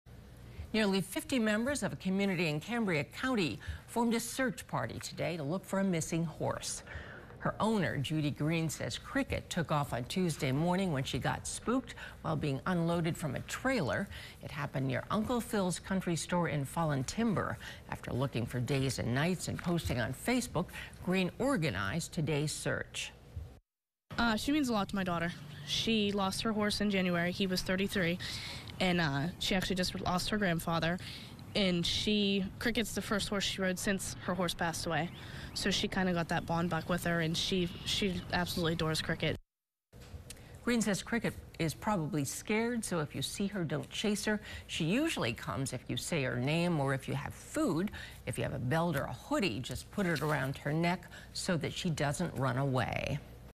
Cambria County community comes together to search for missing horse
Members of a community in Cambria County rallied together Saturday to help find a missing horse.